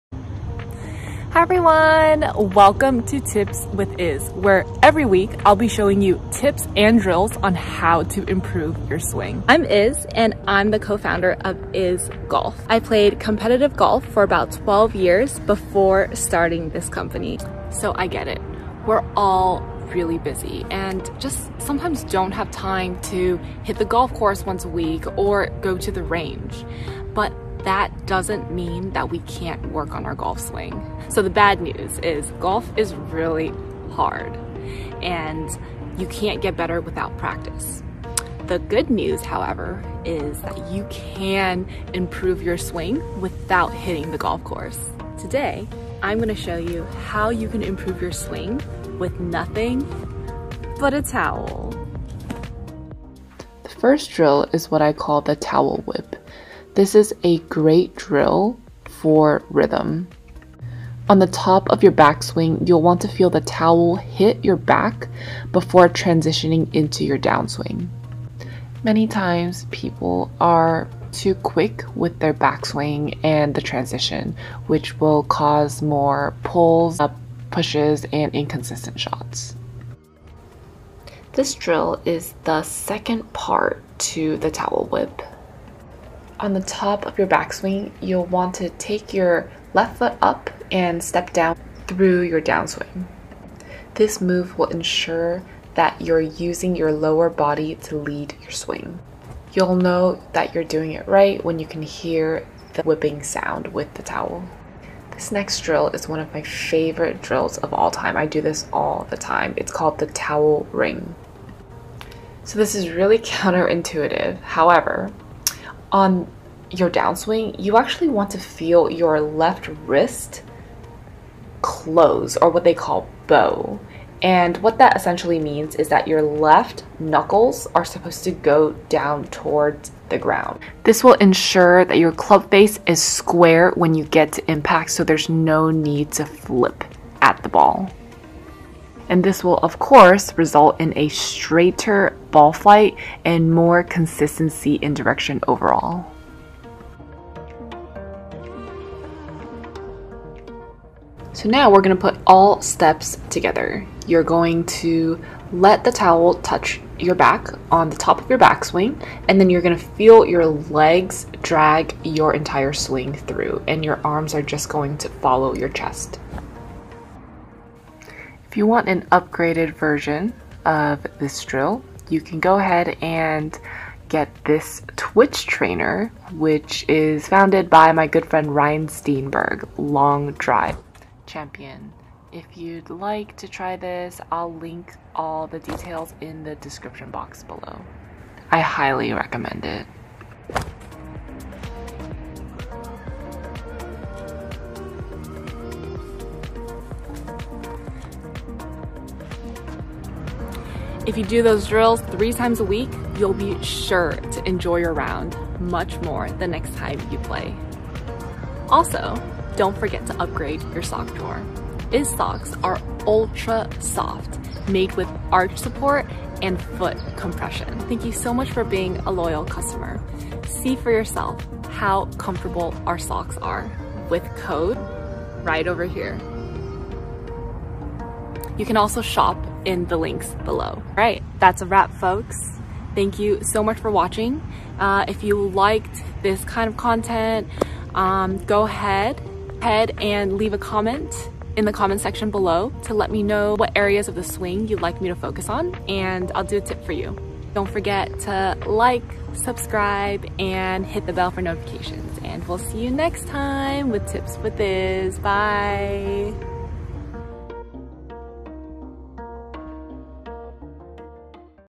Home Golf Drills with Towel - Hit Farther and Straighter
Hi everyone! Welcome back to the channel here, Is here. Today, I'll be showing you three awesome home golf drills with a towel. These drills will target sequence, rhythm and swing position.